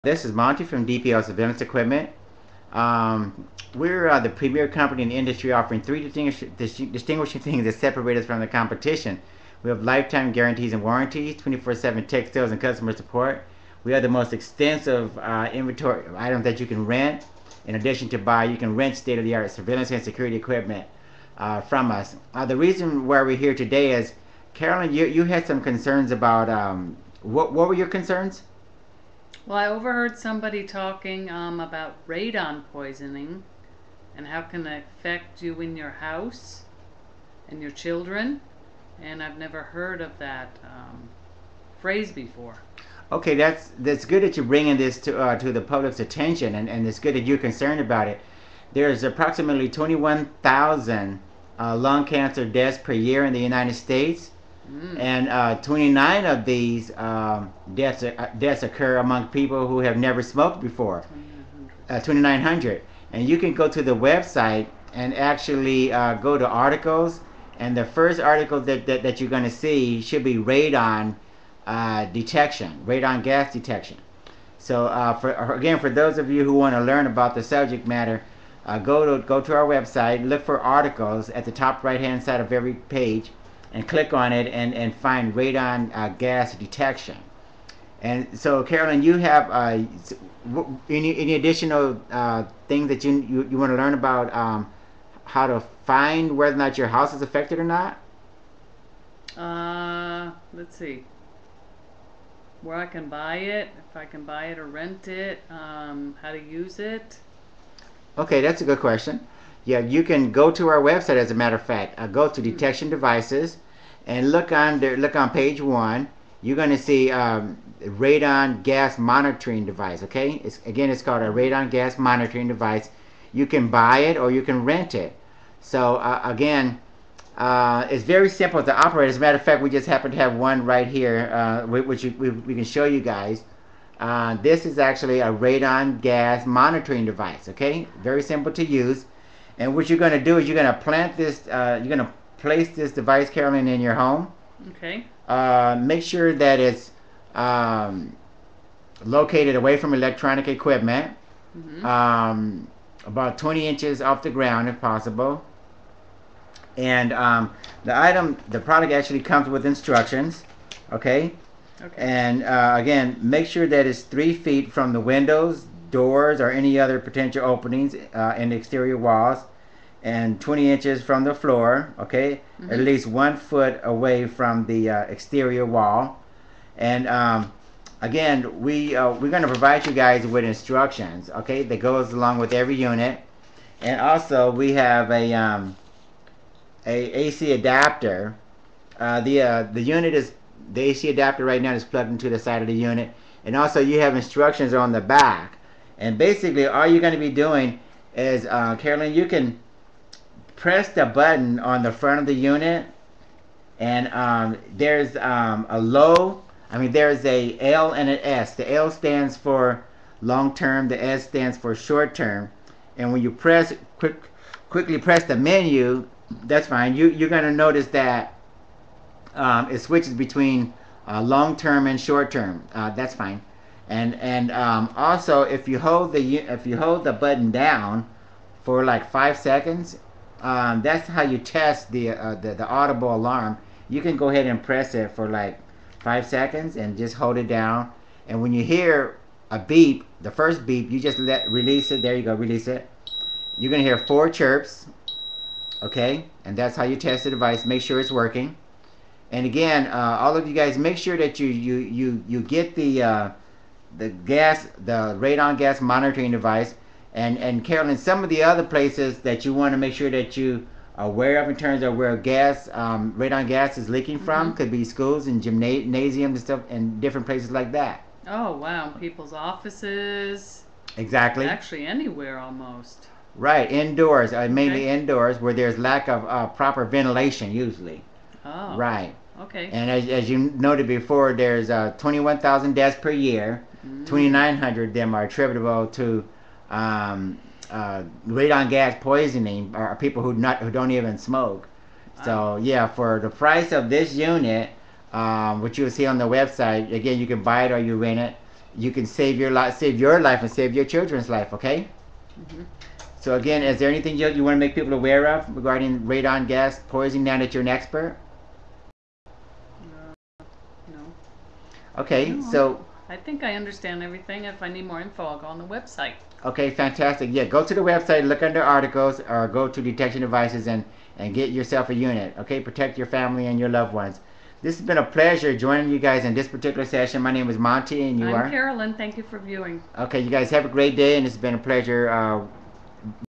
RADON GAS MONITORING DEVICE (Buy/Rent/Layaway)
Protect your family from lung cancer from radon gas exposure with the only EPA evaluated radon gas alarm. Having this monitor in place will provide you and your family peace of mind regarding dangerous radon levels. Radon gas in air or water is a health hazard. Over-exposure to radon can cause lung cancer.